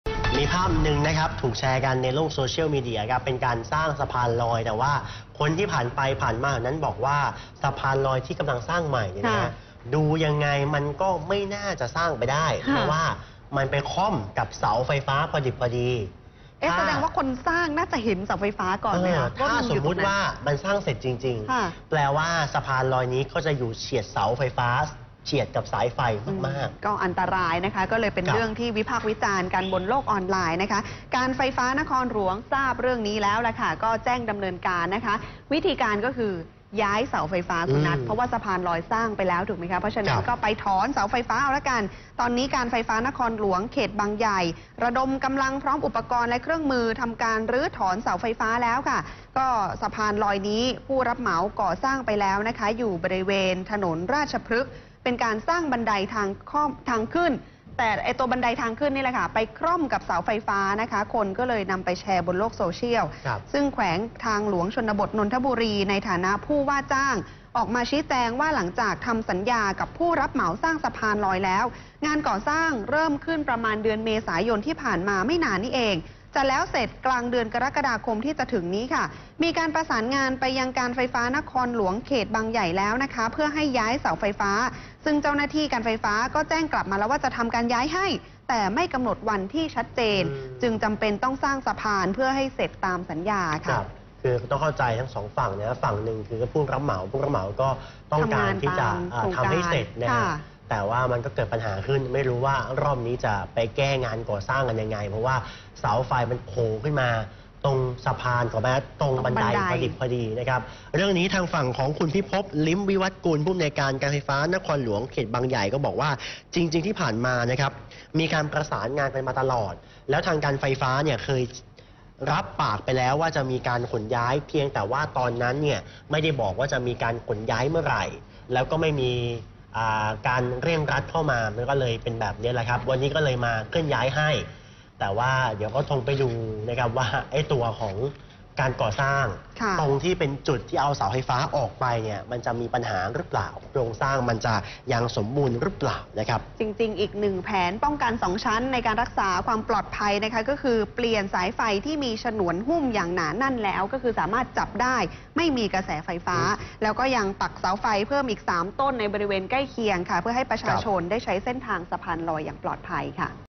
News สะพานลอยเจ้าปัญหา กฟน.บางใหญ่เร่งย้ายเสาไฟฟ้าเรียบร้อยแล้ว
พิกัดข่าวค่ำทั่วไทย : สะพานลอยเจ้าปัญหา กฟน.บางใหญ่เร่งย้ายเสาไฟฟ้าเรียบร้อยแล้ว "NOW26" 13-6-59